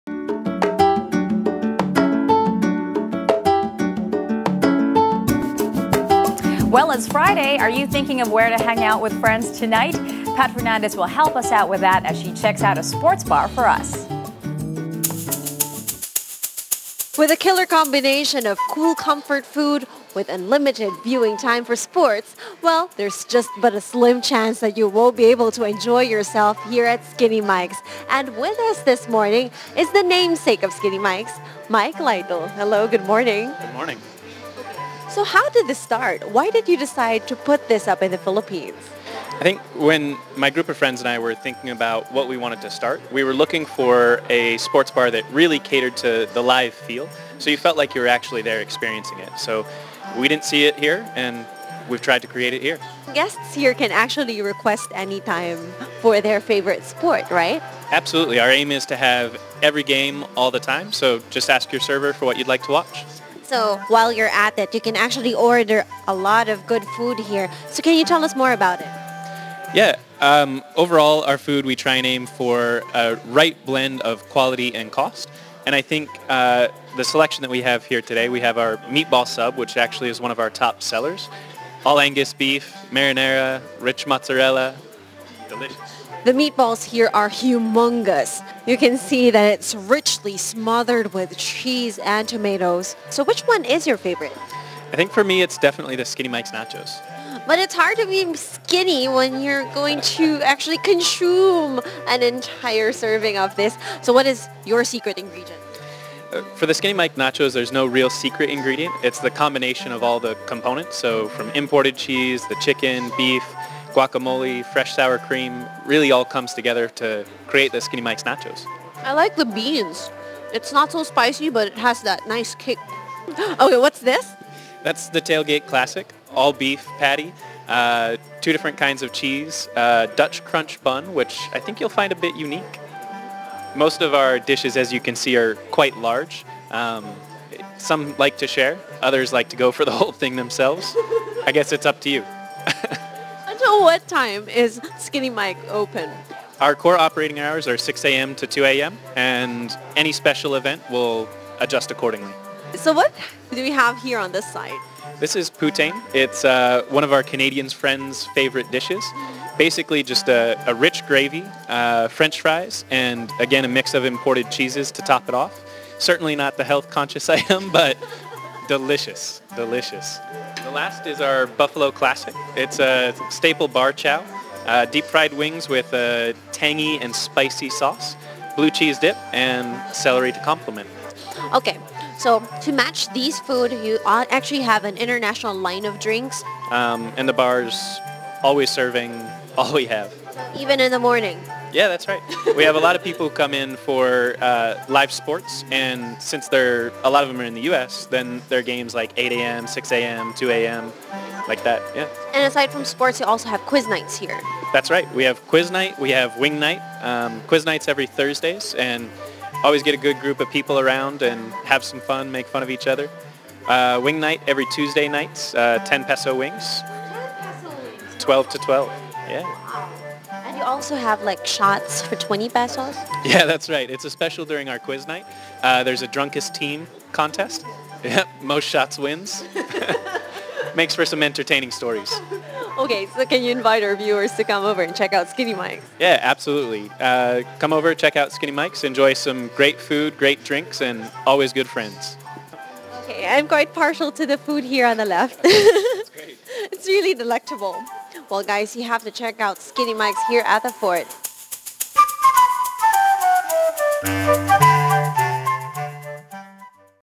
Morning Pick: Skinny Mike's Sports Bar
It's Friday. Thinking of where to hang out with friends tonight?
Pat Fernandez will help us out with that as she checks out a sports bar for us.